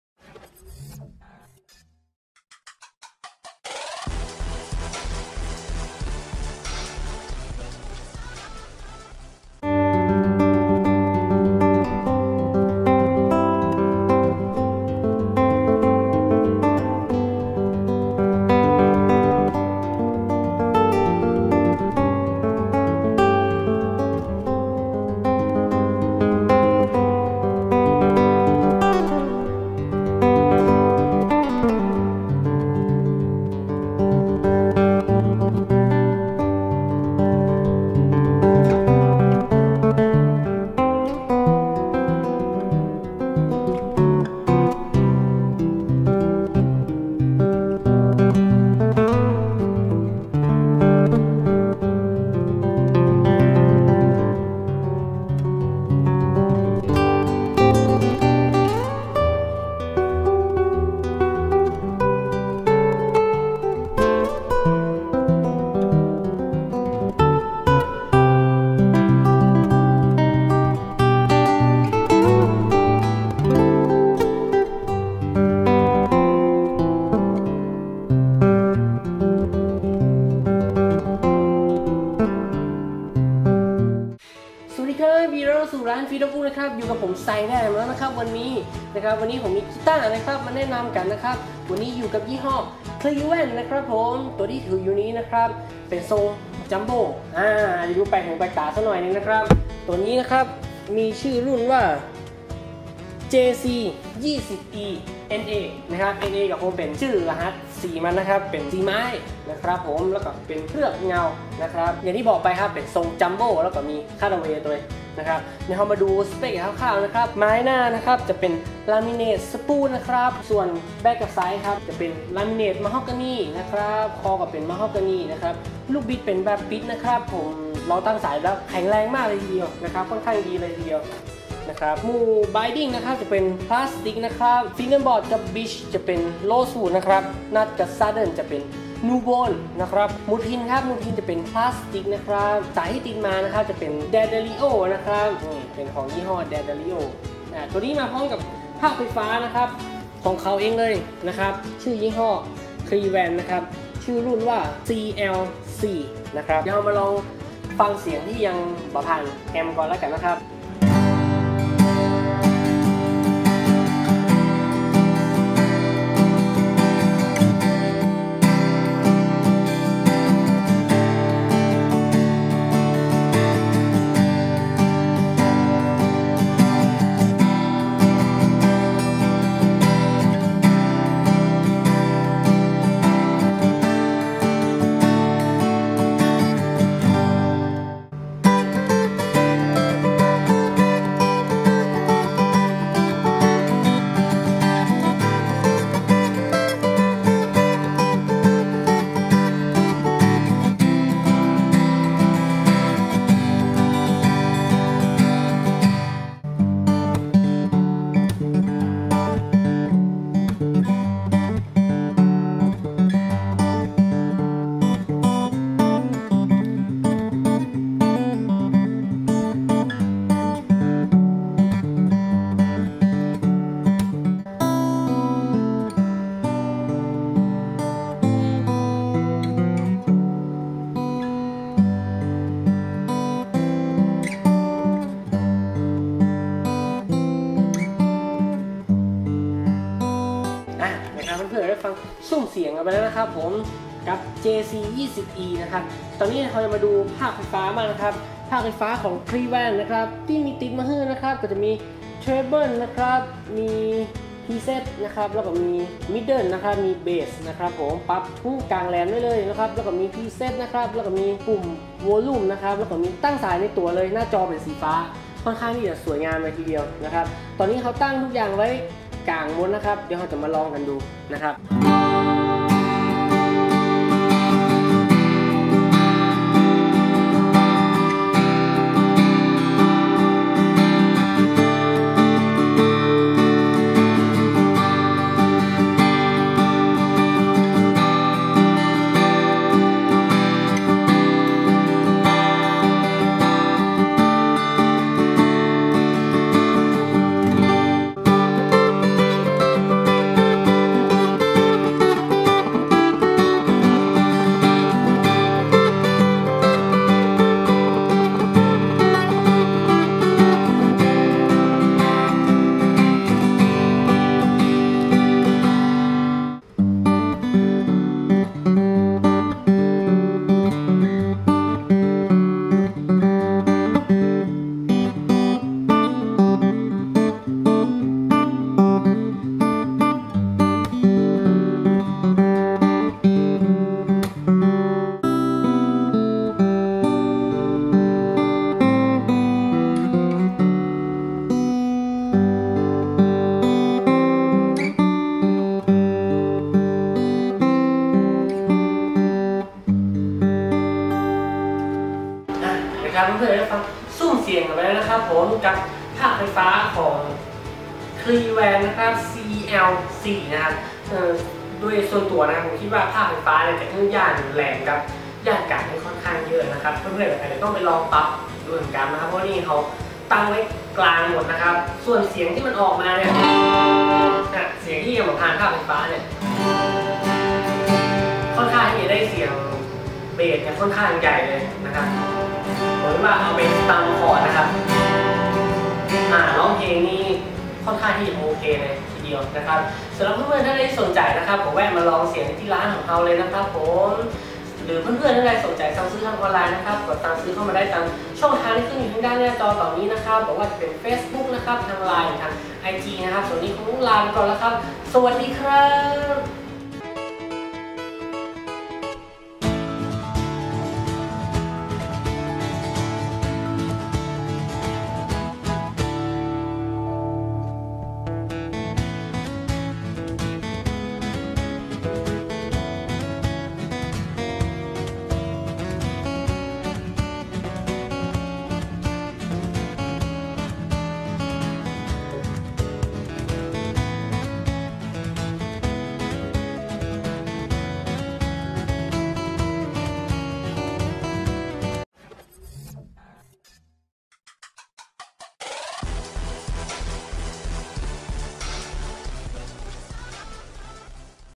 Clevan JC-20E Review by Freedom Uku Music
โทร.087-184-0308

กีตาร์โปร่งไฟฟ้า (Electric Acoustic Guitar) CLEVAN JC-20E

BODY SHAPE : Jumbo Cutaway
xBODY TOP : Spruce
BACK&SIDES : Mahogany
NECK : Mahogany
FINGERBOARD : Rosewood
SCALE : 648mm
MACHINE HEAD : Cover
STRING : D'Addario :EJ10
SADDLE : Nubone
EQ : Design by B-Band:CL-4
HARDWARE : Chrome
COLOR : NA